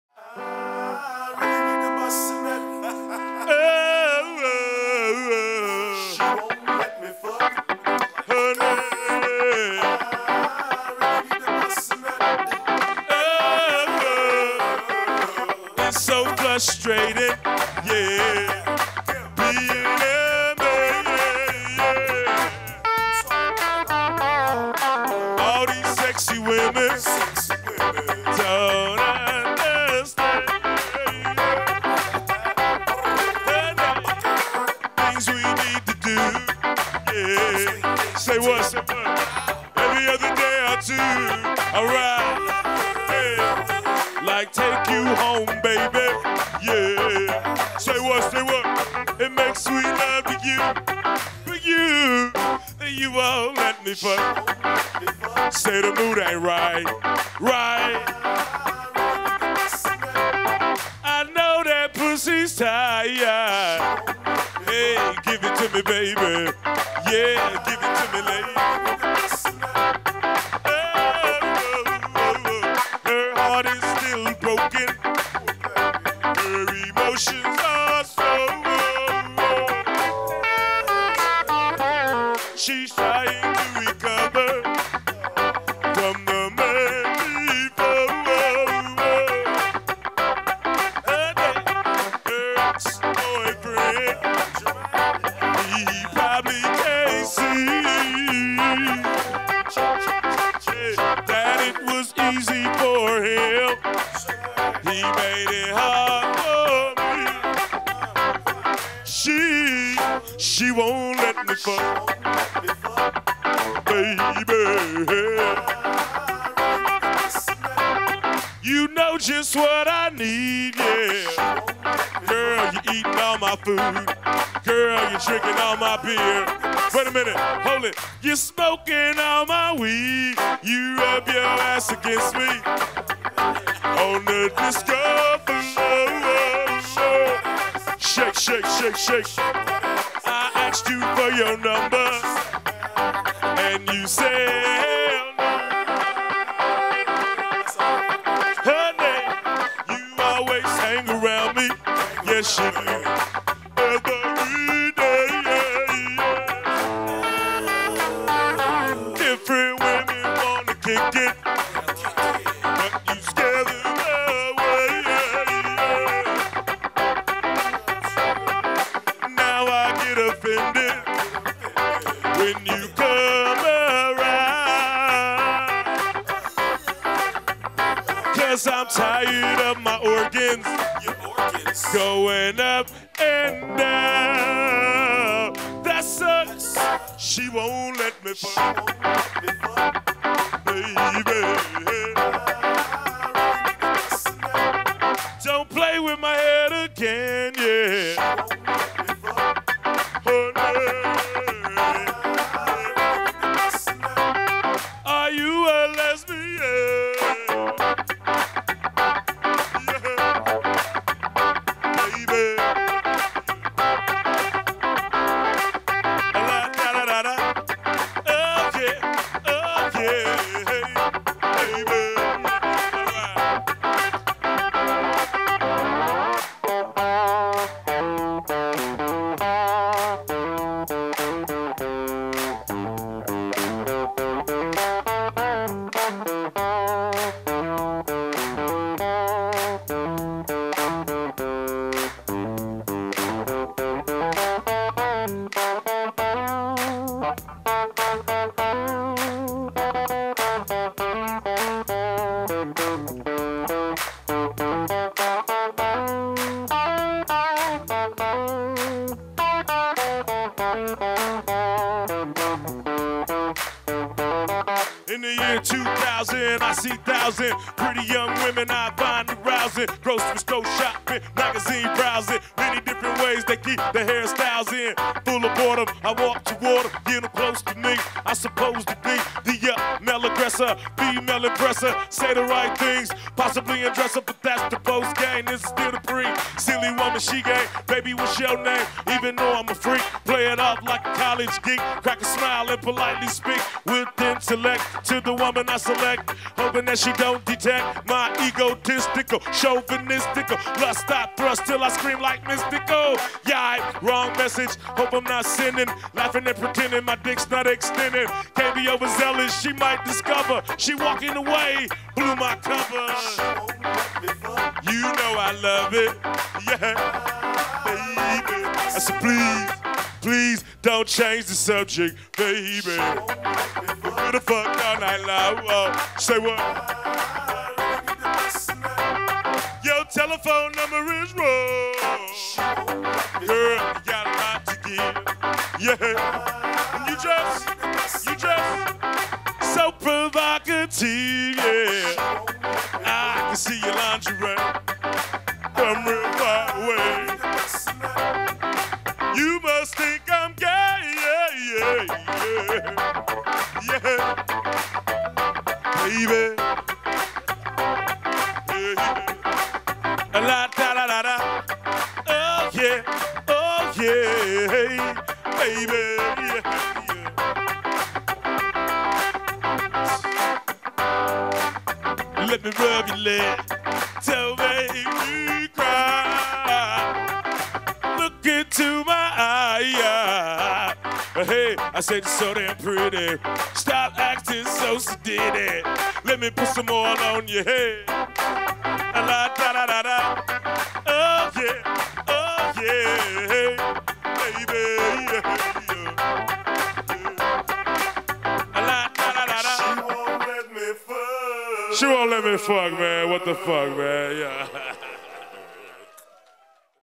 Afroman - Live in San Jose 11
AFROMAN Live @ Avalon Club 4/20/11

Video via DLLD2.com

Produced by Juan Romero and Jose Hernandez
Filmed and Edited by San Saefong